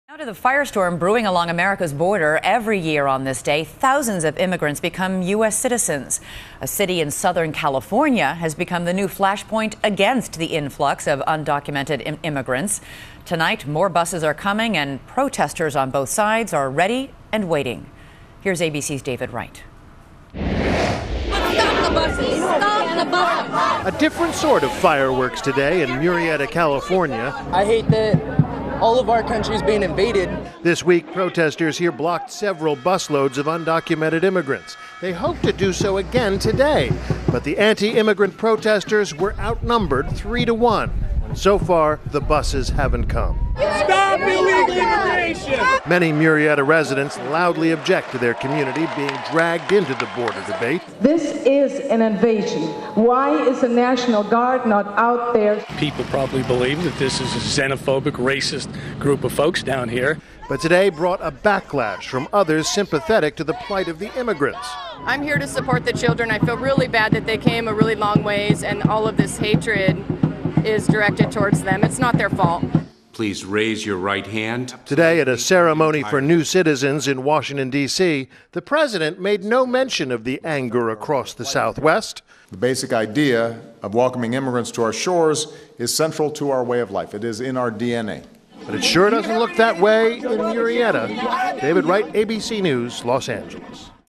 Residents Protest Both Sides of the Immigration Issu
Border tensions on the rise over large number of immigrants, particularly children, pouring over the American border.